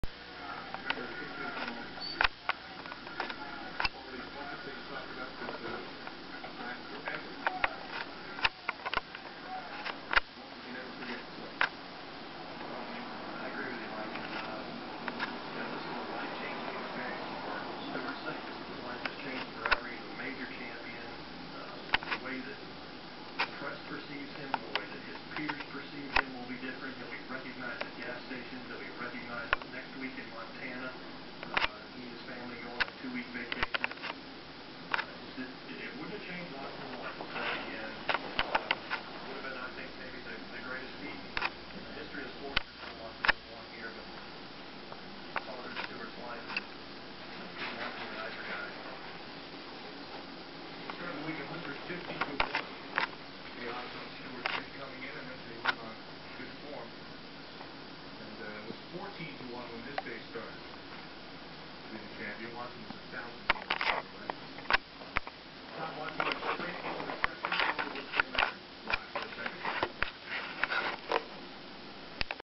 *RARE* Raccoon Sleeping On Dog
Orphan raccoon becomes best friends with beagle.